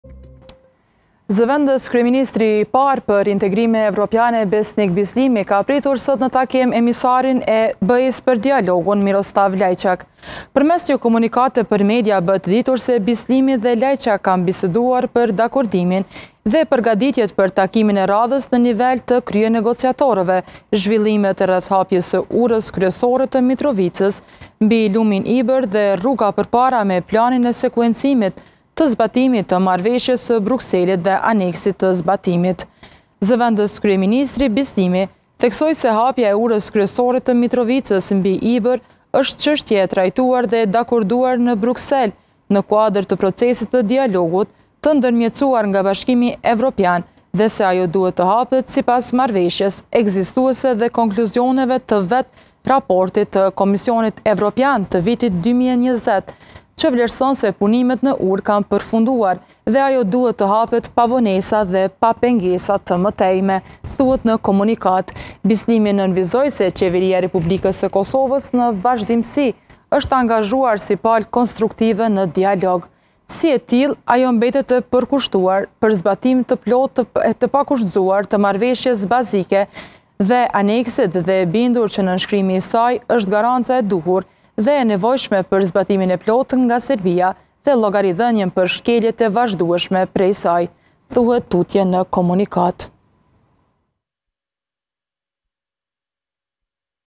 Bislimi-Lajçakut: Hapja e Urës së Ibrit duhet të bëhet pa vonesa
🔴 Raport - Aksionet në veri, serbët paralajmërojnë bllokada - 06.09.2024